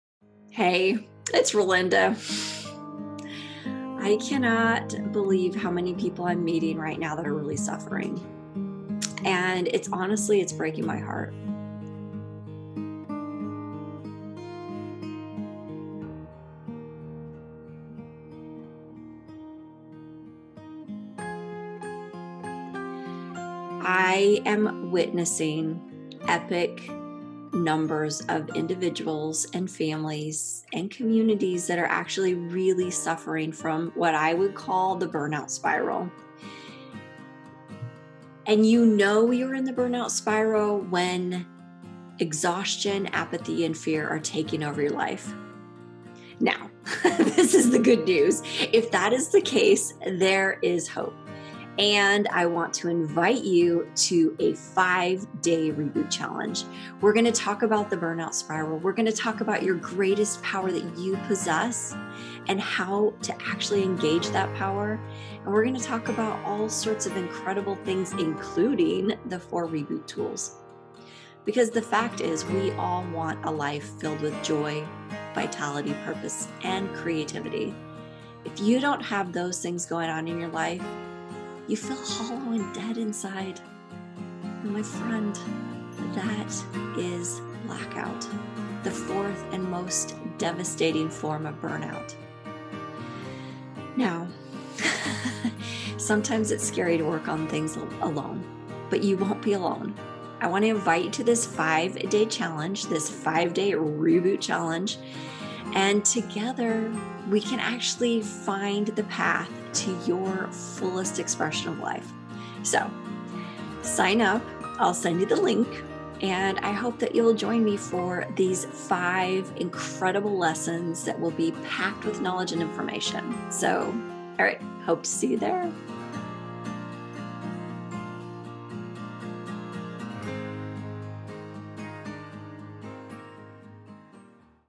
Overcome Burnout Challenge - 5 Day Reboot
Are you suffering for burnout?  Times are really hard...has exhaustion, fear, anxiety, apathy, or hopelessness taken over your life? Take the 5 Day Reboot Challenge and Reboot your life.